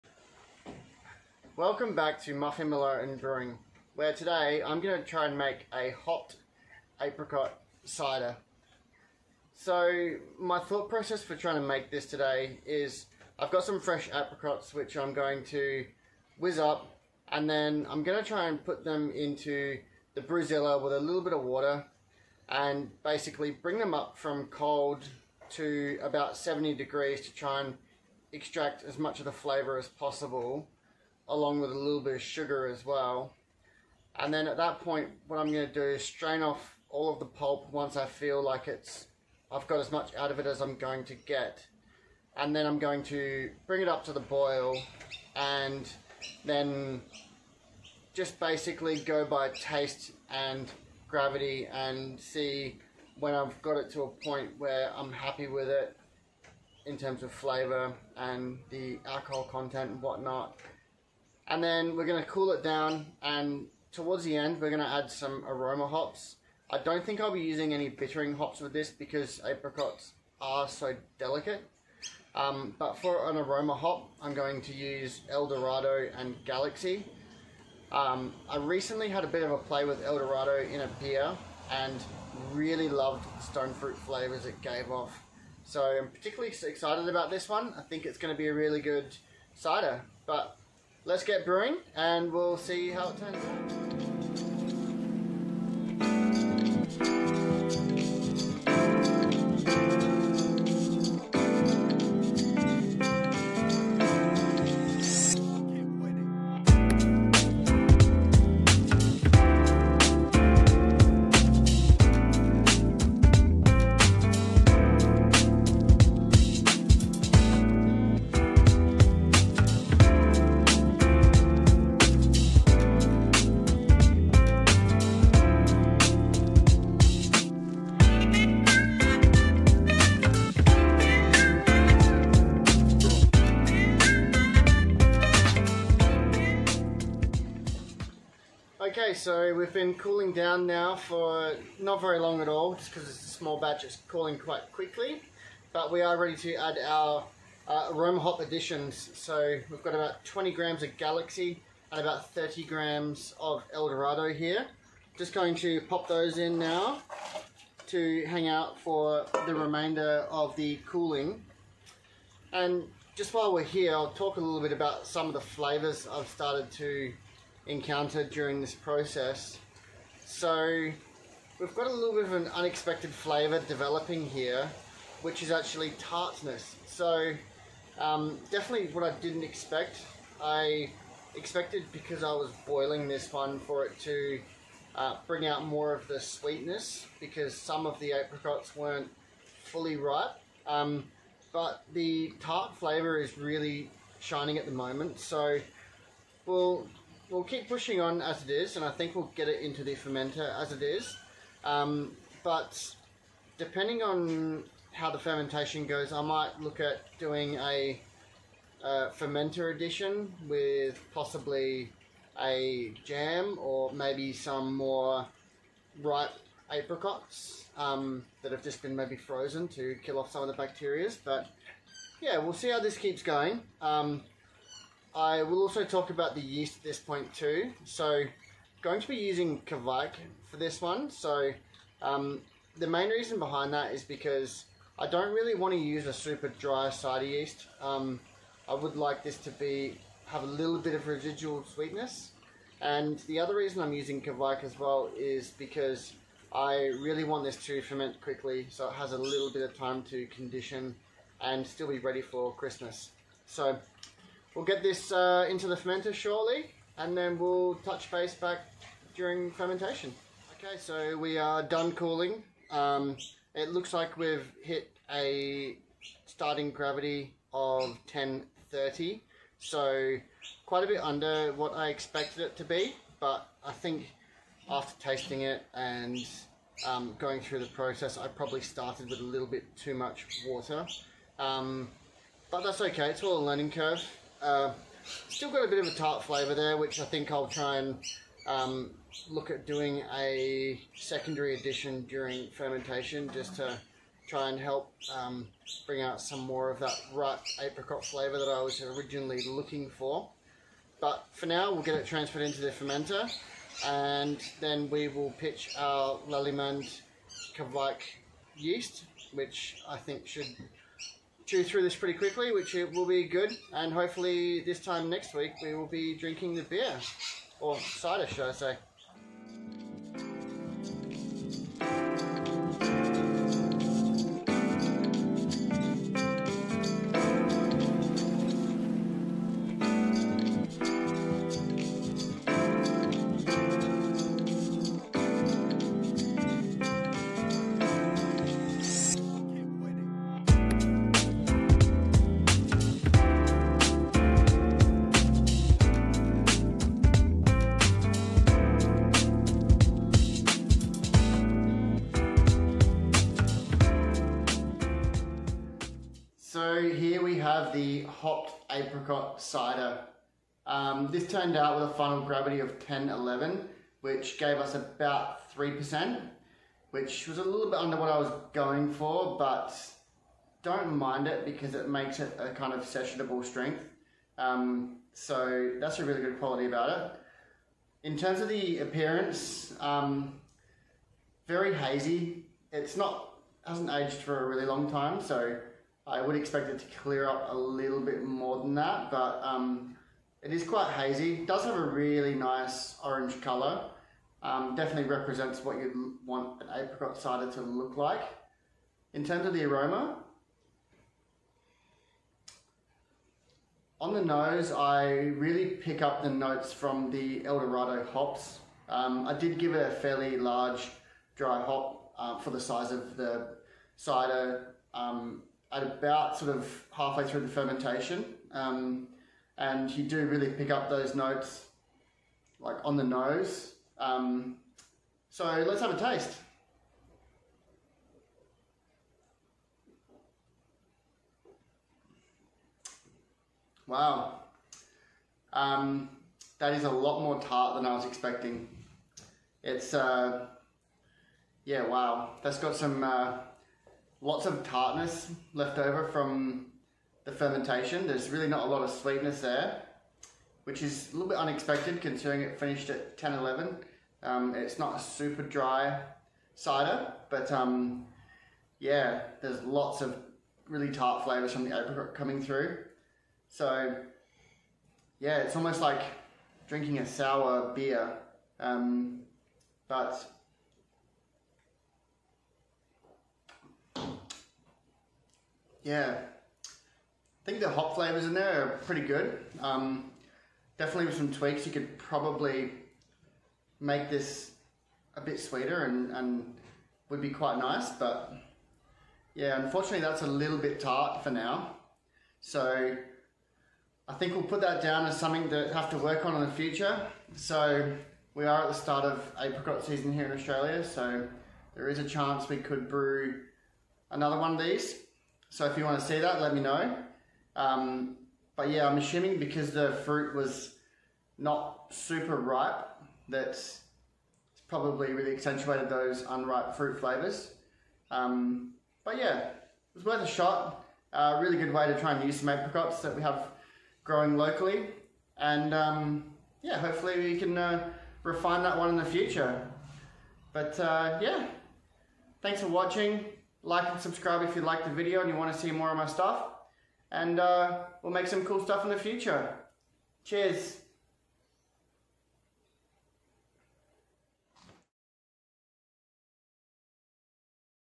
HOPPED APRICOT CIDER. my first attempt
join me as I attempt my first hopped apricot cider.
each year I watch countless local apricot trees get desimated by birds. this year I was determined to make a cider with foraged local fruits.

for the cider I used

8L water
3kg fresh De seeded apricots
500g sugar

bring from cold water to a boil then strain pulp and cool the liquid .

hops
20g galaxy hop stand @75°c
30g eldorado hop stand @75°c

ferment with lallemand keviek Voss @30°c

30g eldorado dry hop for 3 days

equipment

brewzilla 35 L

keg king junior fermenter